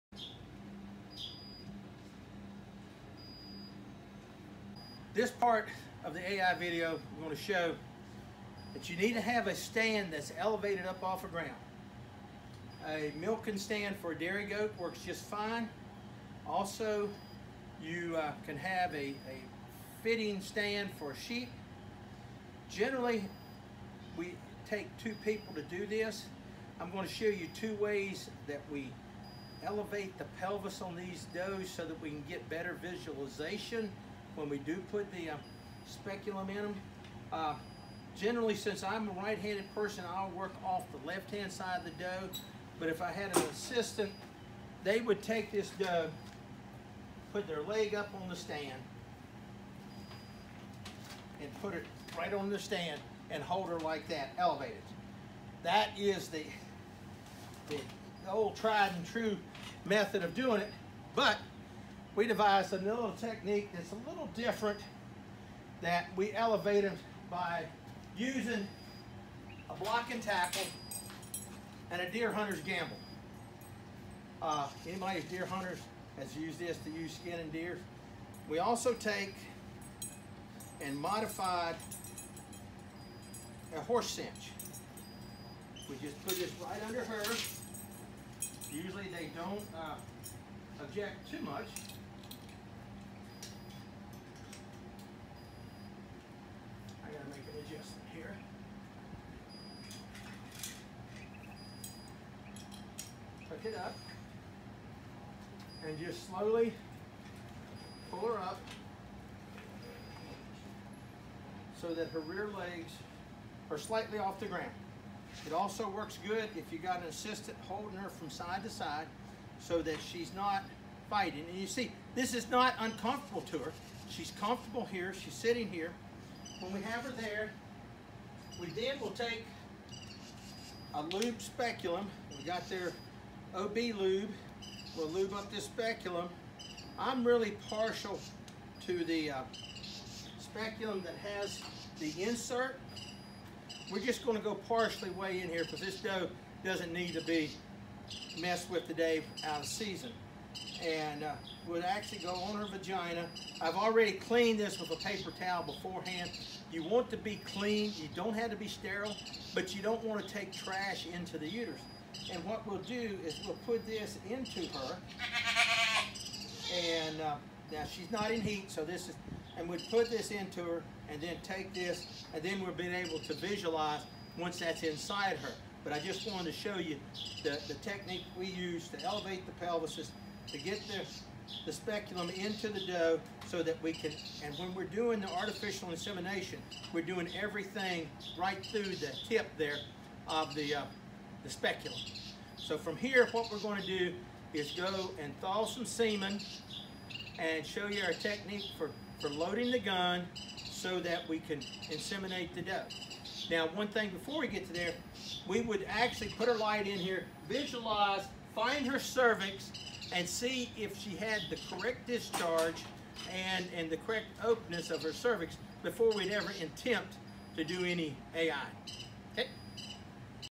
Small Ruminant Insemination Techniques
Videos from NC Cooperative Extension, Edgecombe County Center

Part 2 of the Small Ruminant Artificial Insemination Series with Large Animal Veterinarian, Dr. Cole Younger.  In this video, Dr. Younger goes over how to prep a doe, and how to mount her securely.